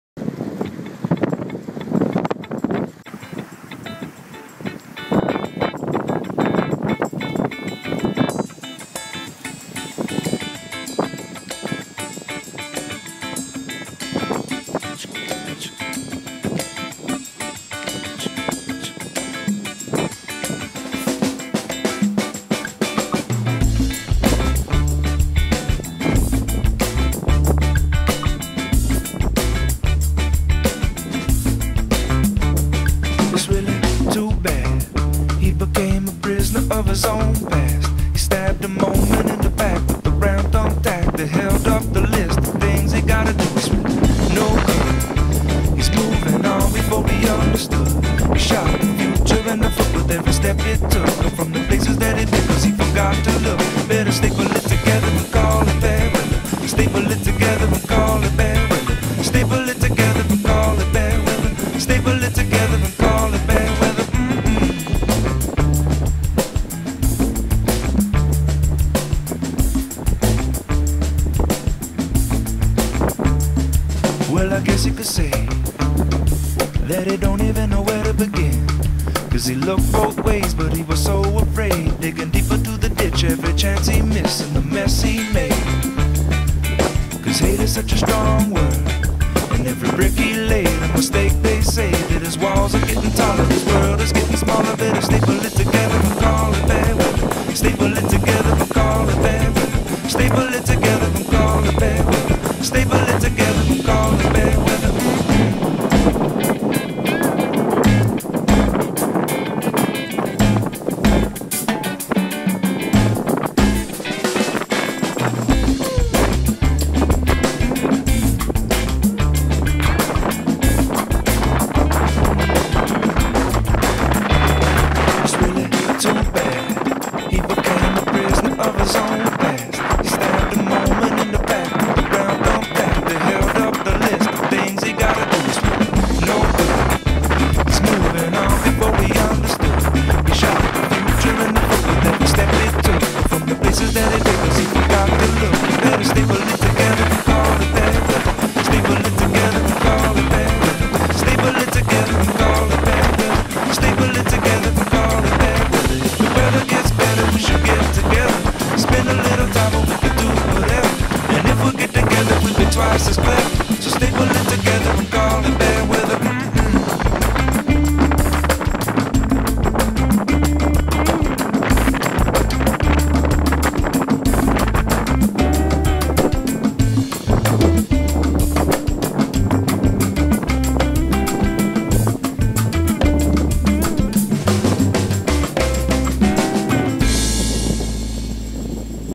Stranger film
De motorboot Stranger, gebouwd door Jan de Ruiter uit Lelystad, vaart op het IJsselmeer in de zomer van 2012.  Deze Range boat is een Nigel Irens ontwerp. Hij is gebouwd met glas en epoxy.